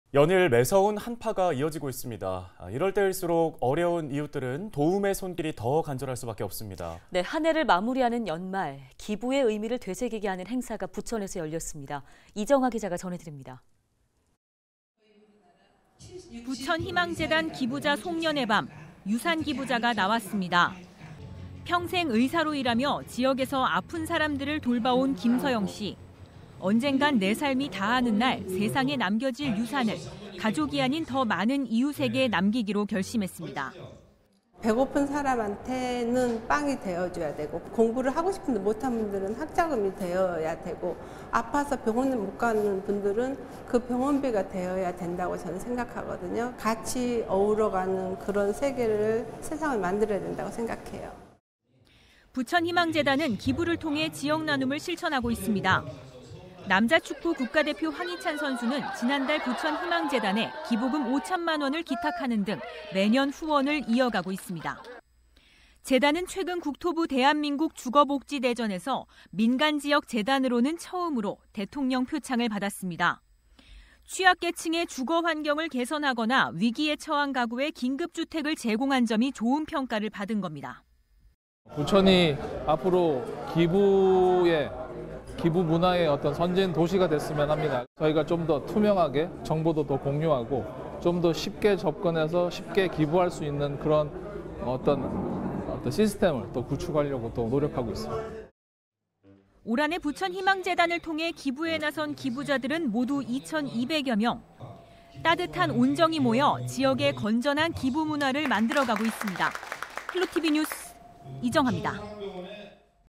나눔과 기부 확산…부천희망재단 '훈훈 연말' l
연일 매서운 한파가 이어지고 있습니다. 이럴 때 일수록 어려운 이웃들은 도움의 손길이 더 간절할 수밖에 없는데요. 한해를 마무리하는 연말, 기부의 의미를 되새기게 하는 행사가 부천에서 열렸습니다.이정하 기자의 보도입니다.